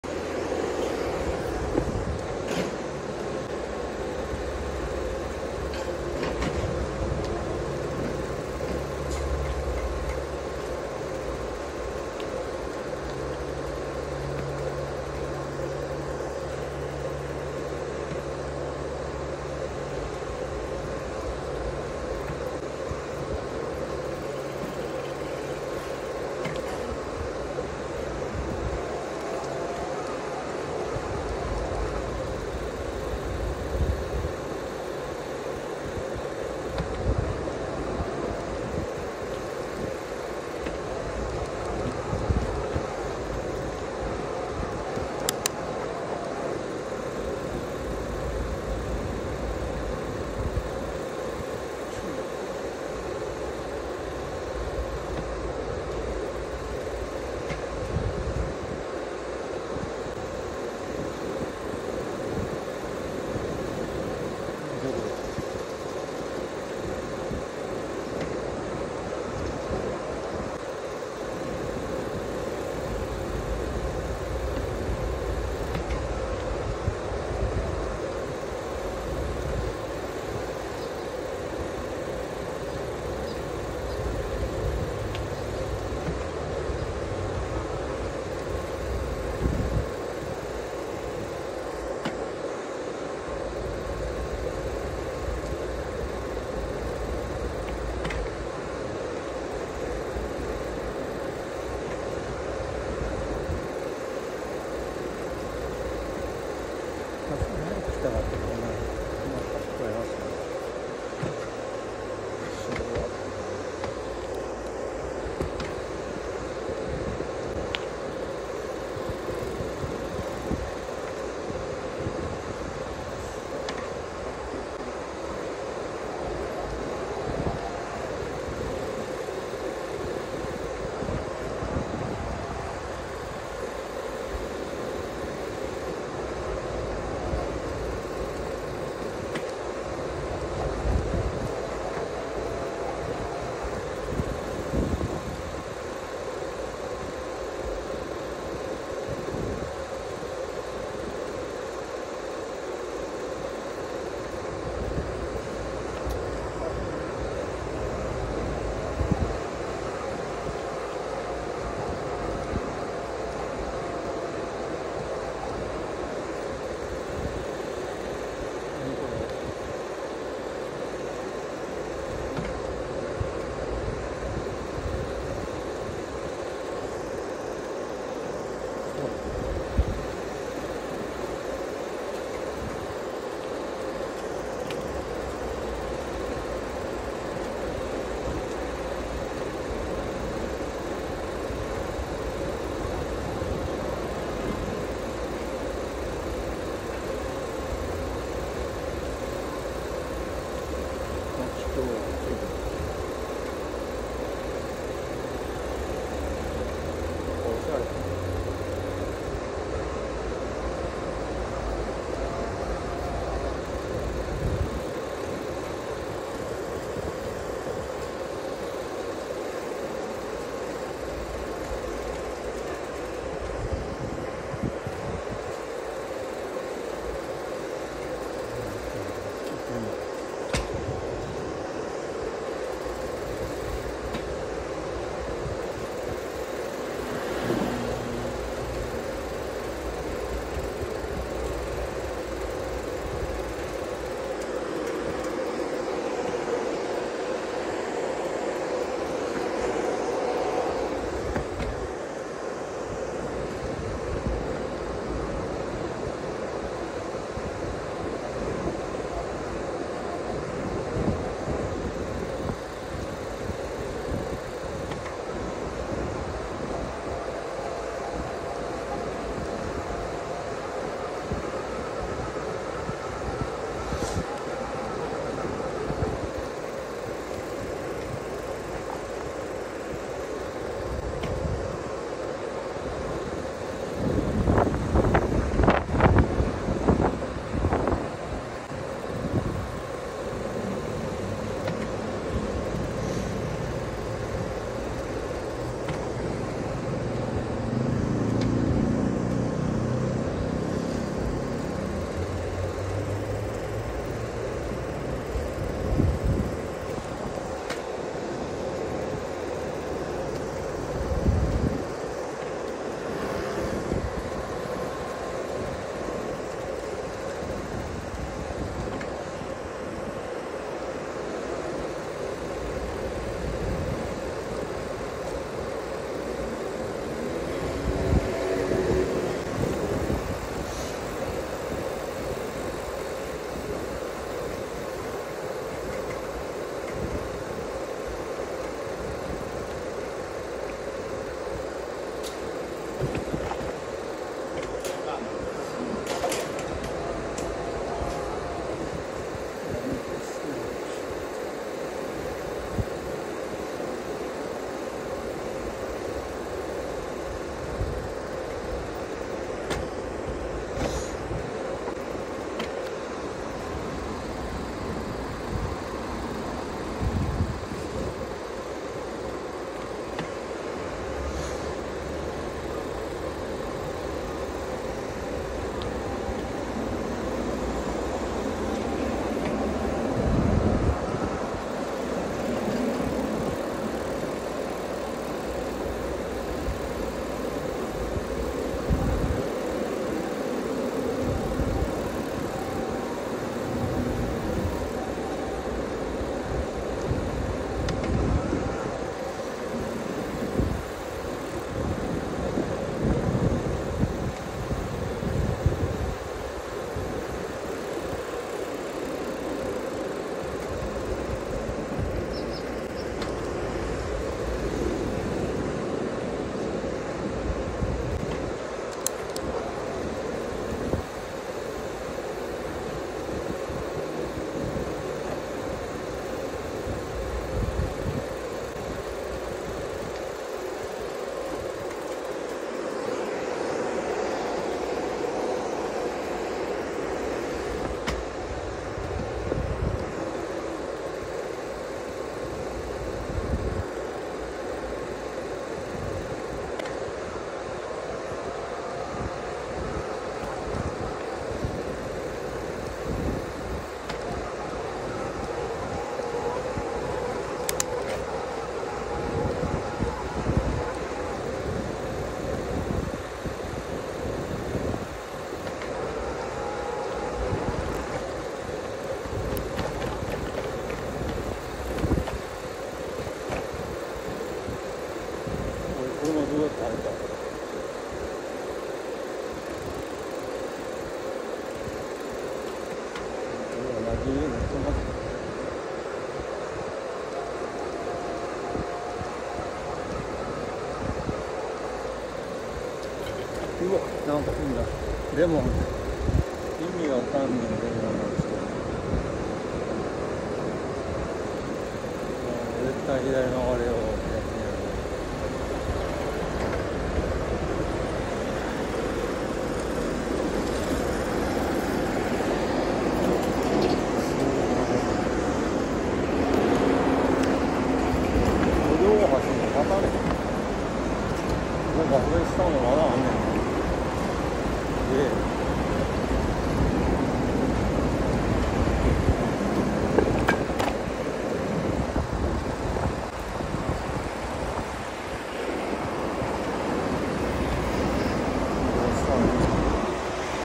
【下京区】正面橋から河原町五条らへんを散策する動画【高瀬川】
高瀬川周辺を散策。